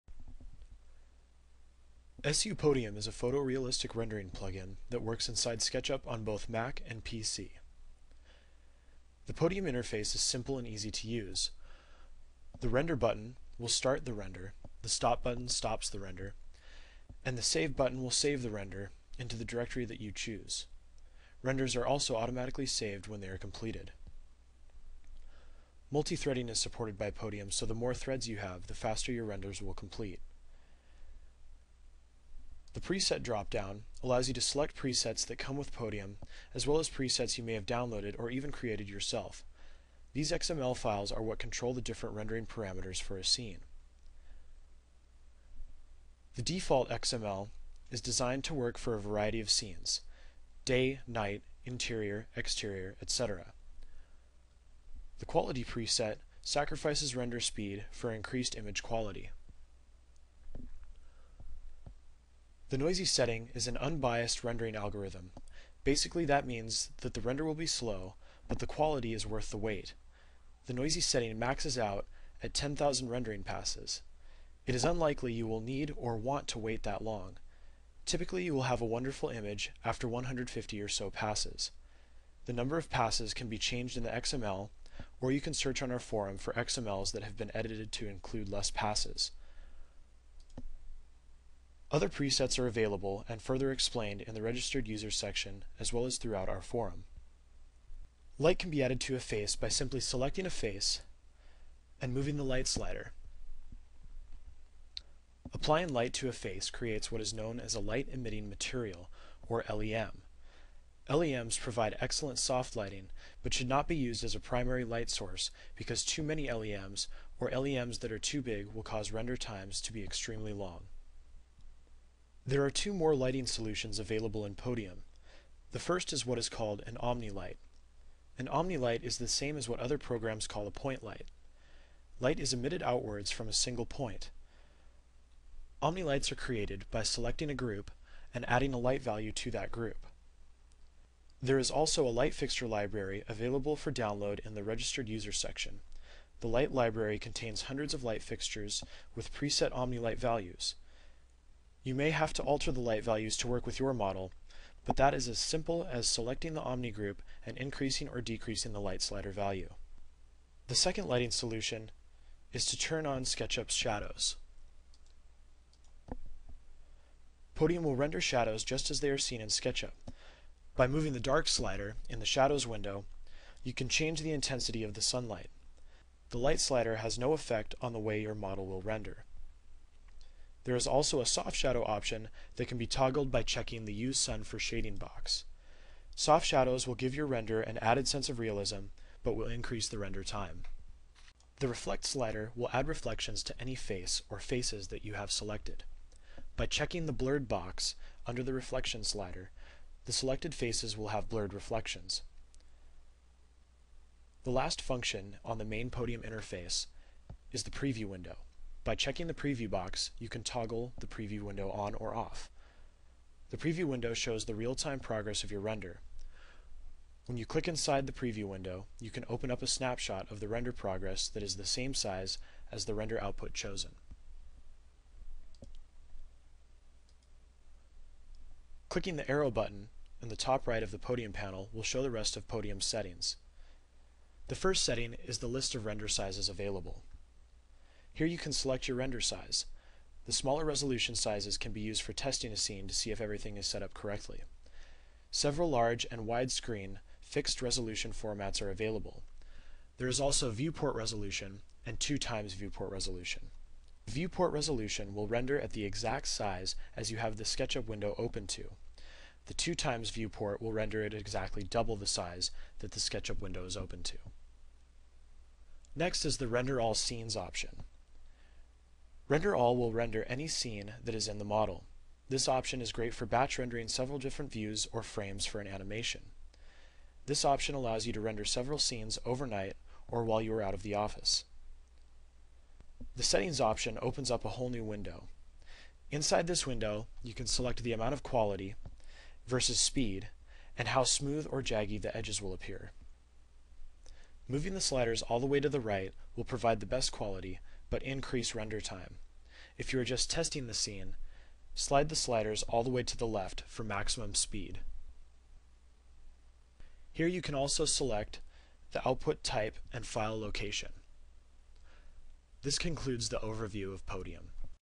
SU Podium Overview
SU Podium is a photo-realistic rendering plugin that works inside Google SketchUp on both Mac and Windows operating systems. for more information.